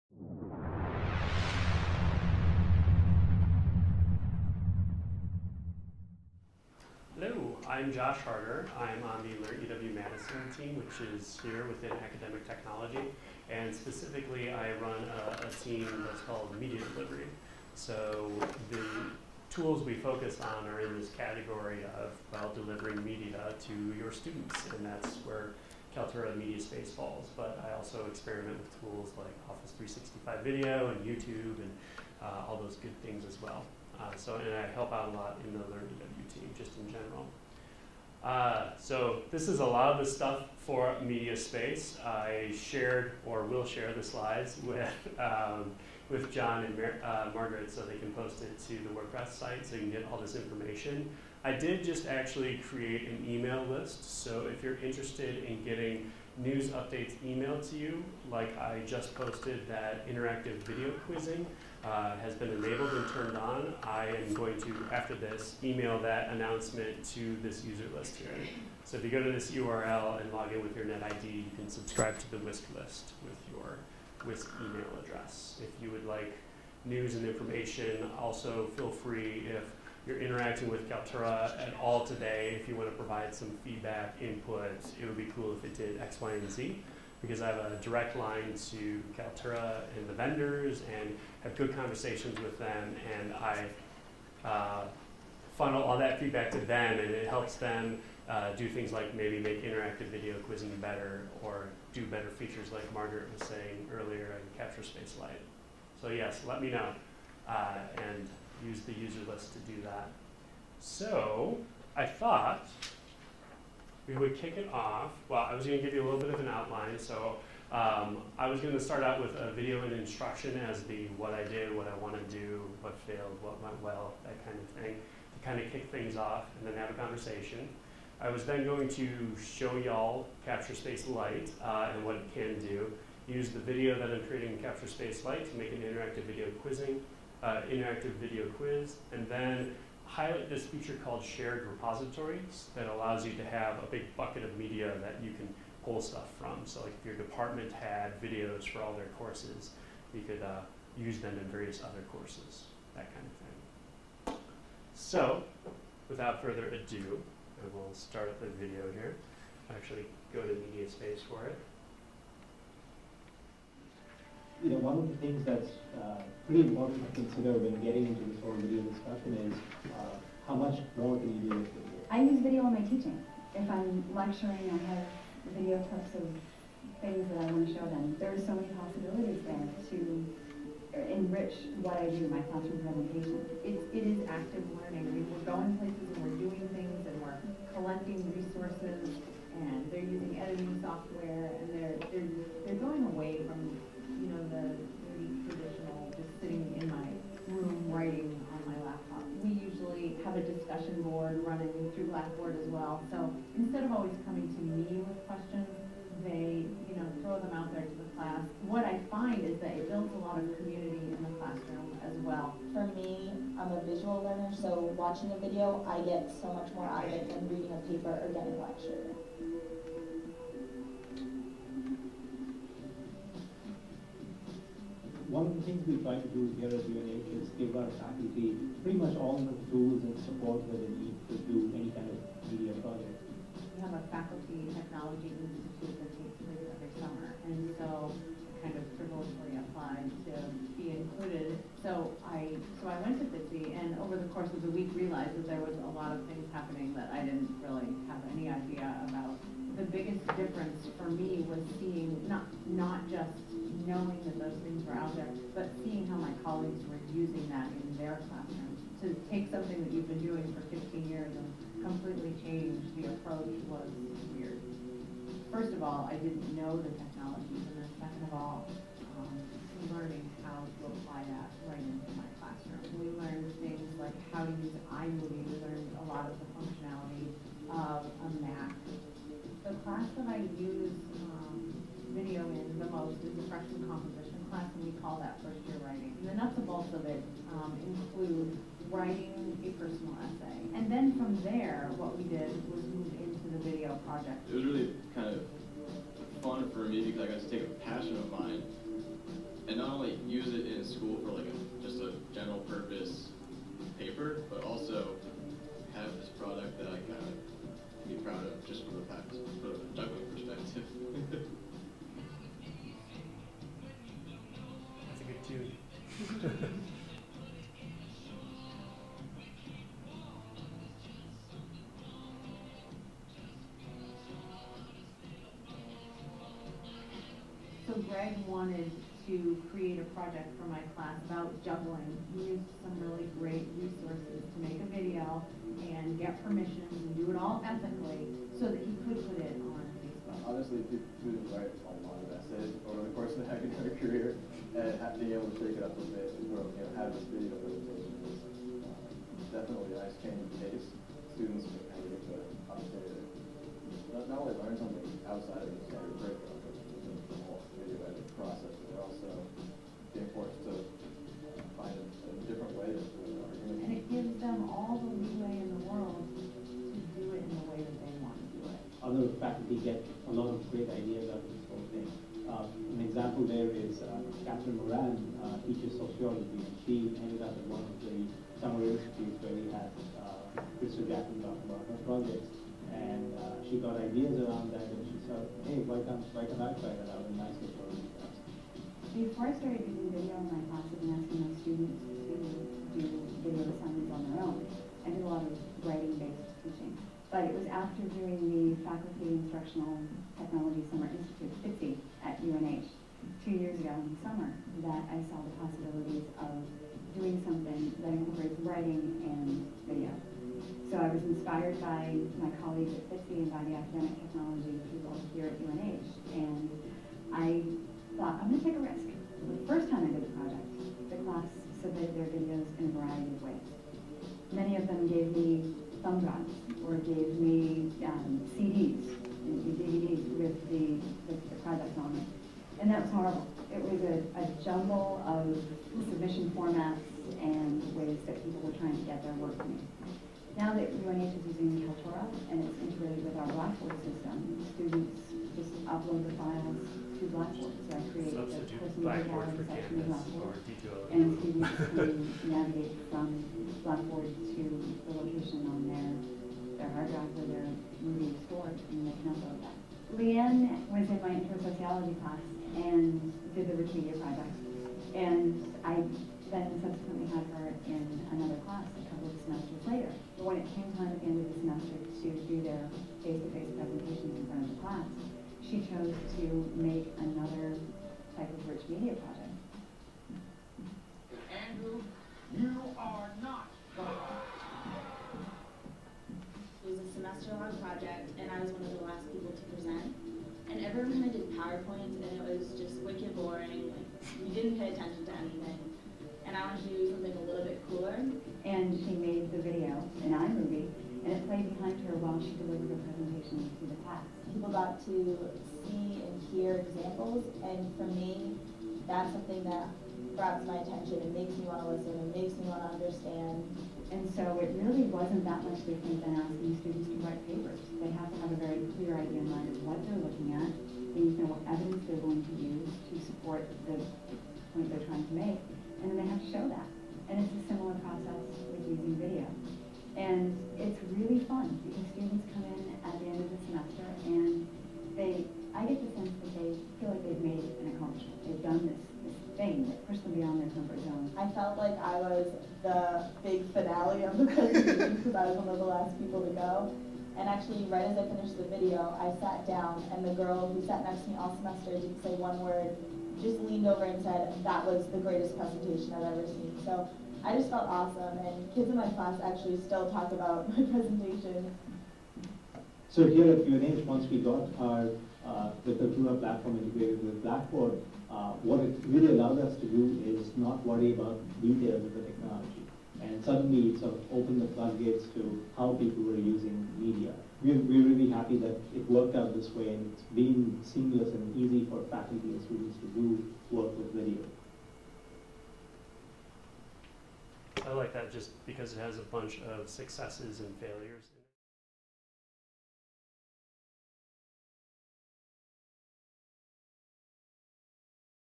[Story] Kaltura MediaSpace with Josh Harder (Apr 6, 2016)
Josh Harder from DoIT Academic Technology shares how to used Kaltura MediaSpace, a social video and rich media portal. Recently added, Interactive Video Quizzing (IVQ) lets instructors embed short multiple-choice quiz questions in videos as knowledge-checks for student learning.